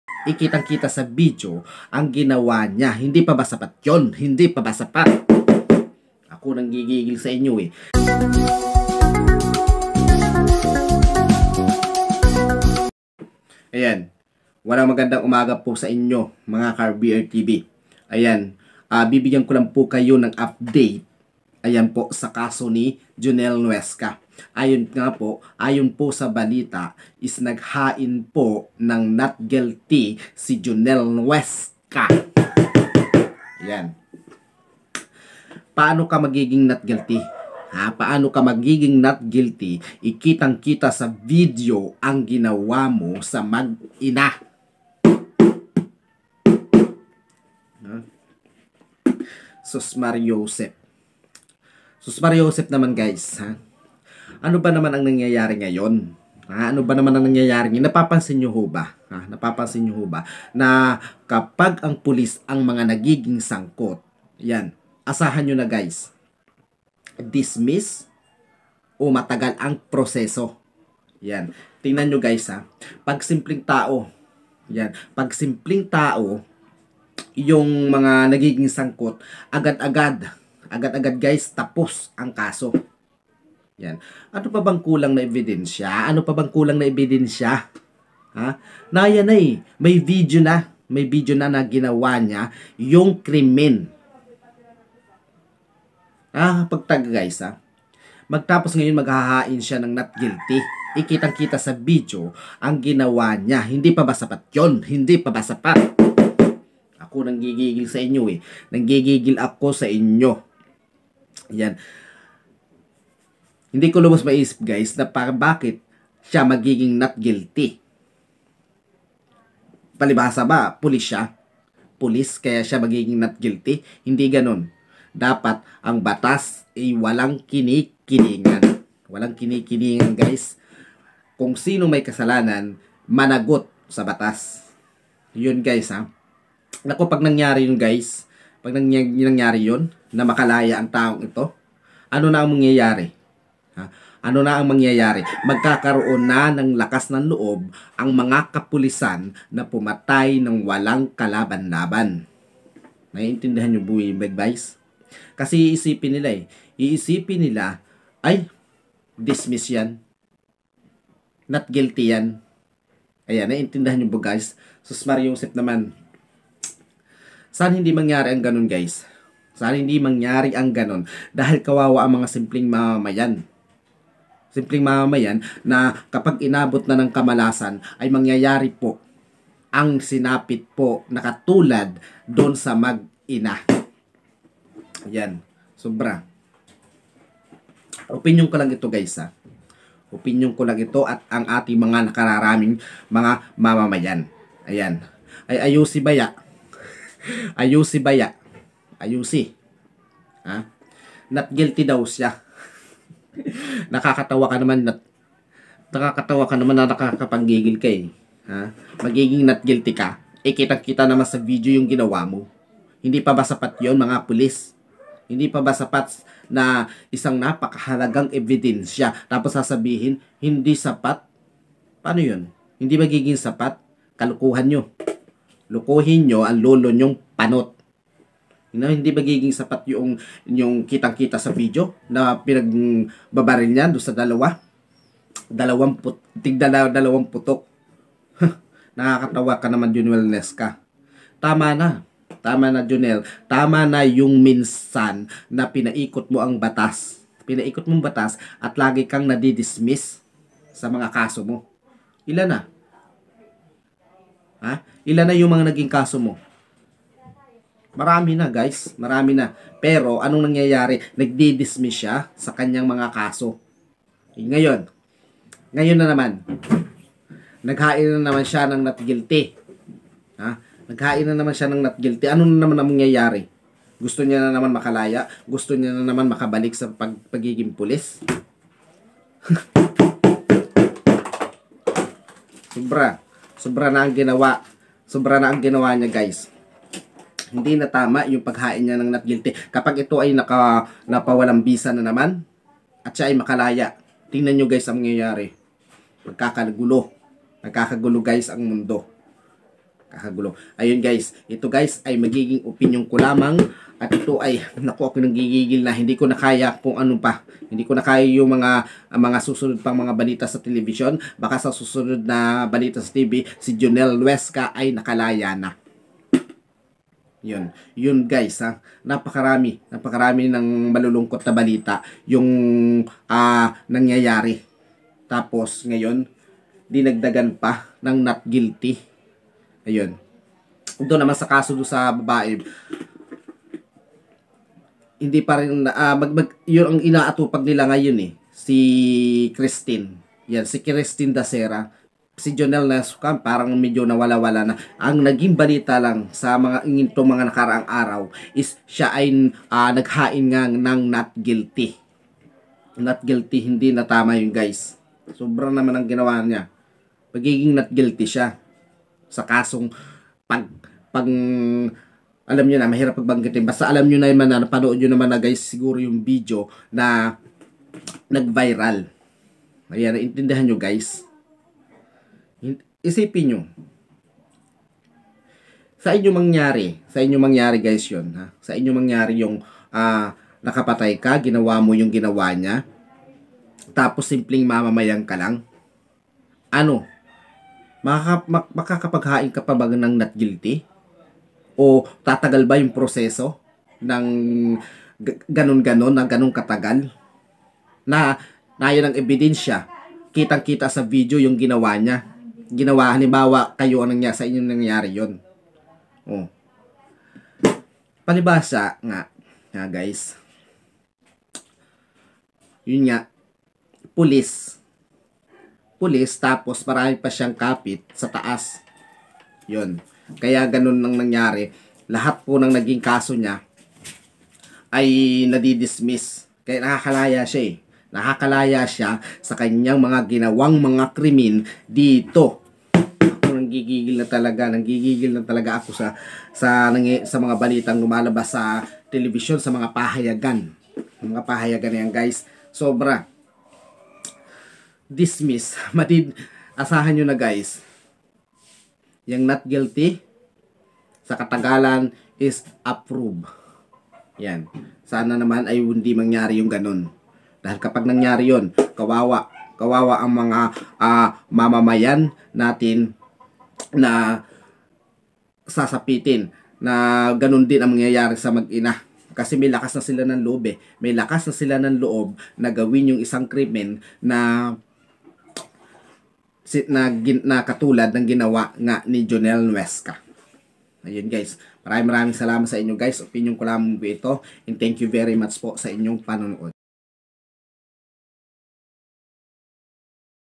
SINO BA ANG MAY SALA
isang balita ang kumalat na si jonel nuezca ay nag hain na ng not guilty. panoorin natin guys.